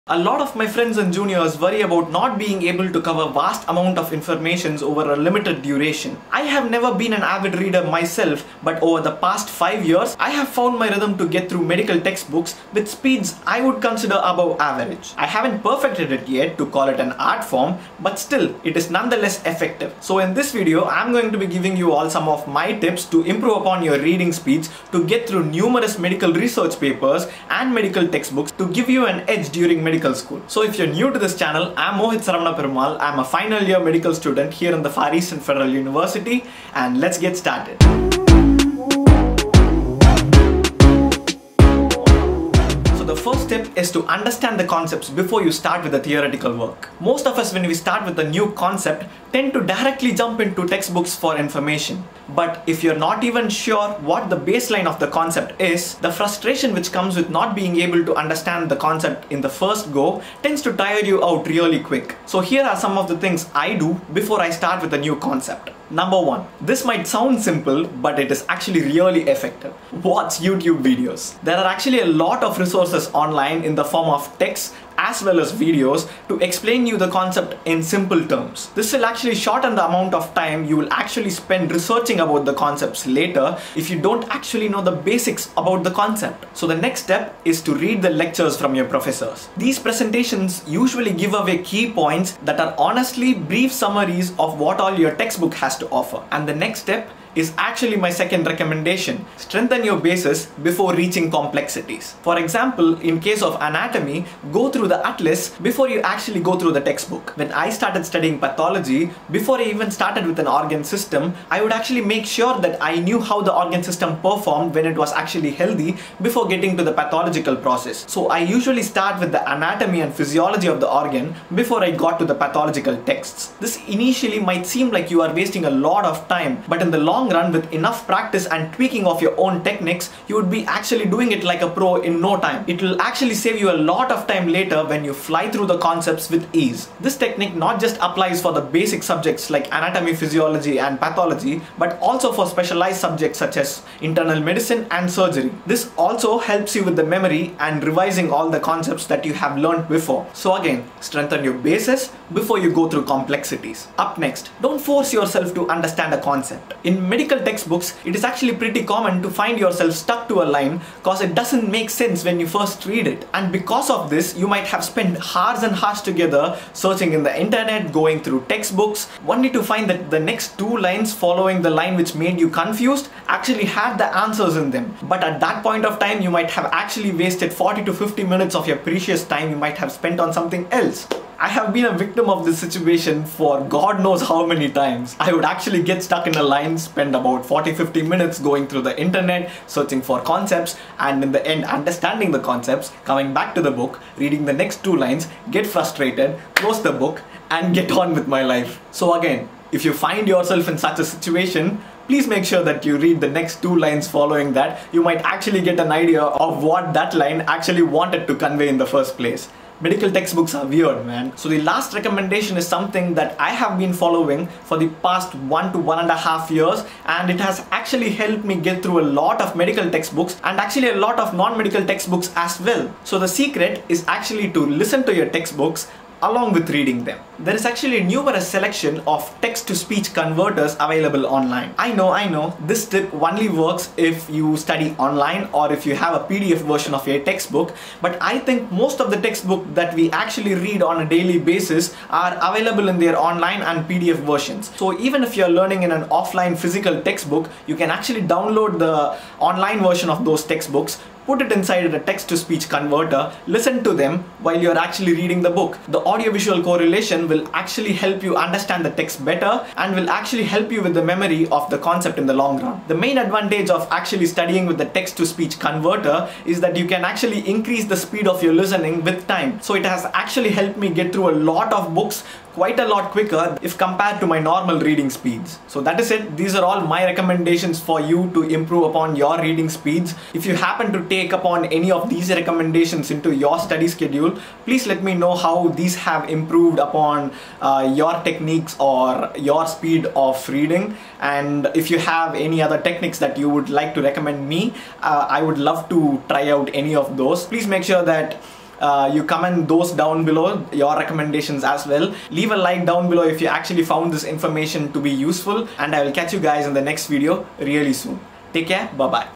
How To Study More in Medical School I Study Tips I Far Eastern Federal University I
So in this video am going through few tips that have helped me read numerous medical textbooks and scientific papers faster and effective in medical school.

Time Stamps:
1. Introduction - 00:00
2. Understand the Primary concepts - 00:40
3. Things to Do before starting a new Concept (step 1) - 01:01
4. Things to Do before starting a new Concept (step 2) - 01:25
5. Strengthen your basics before complexities - 01:39
6. Don't Force yourself into understanding a concept - 02:33
7. A little secret - 03:31

Mohit Saravana Perumal,
Far Eastern Federal University.